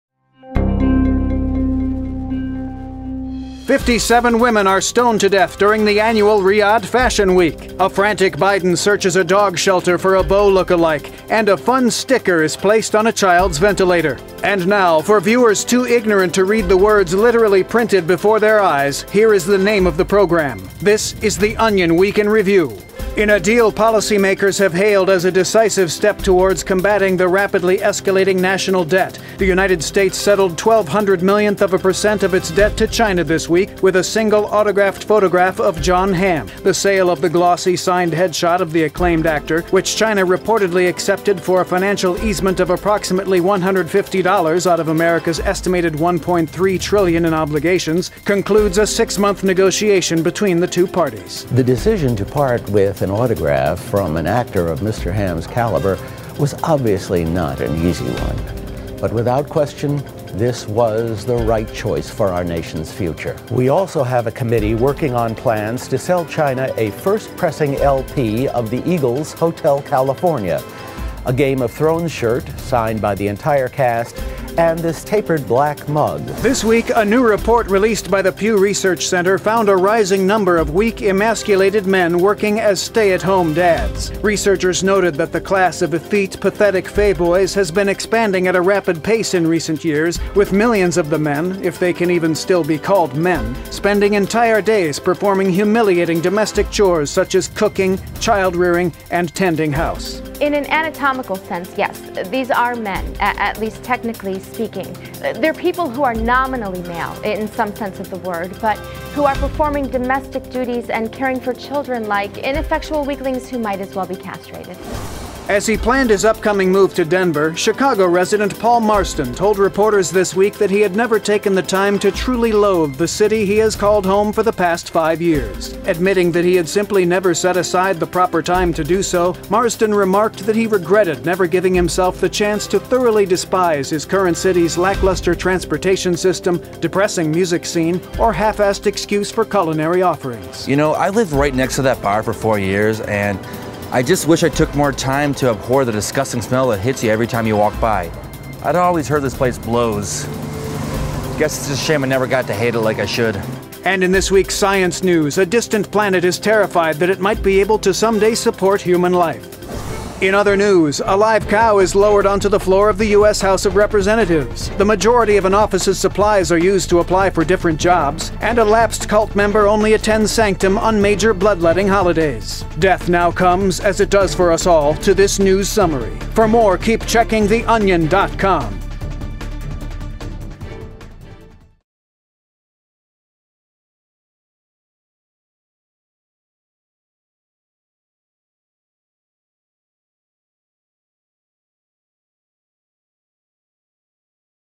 United States Settles .000000012% Of China Debt With Autographed Photo Of Jon Hamm
57 women are stoned to death during the annual Riyadh fashion week, a frantic Biden searches a dog shelter for a Bo look-alike, and a fun sticker is placed on a child's ventilator. It's the week of February 6, 2014.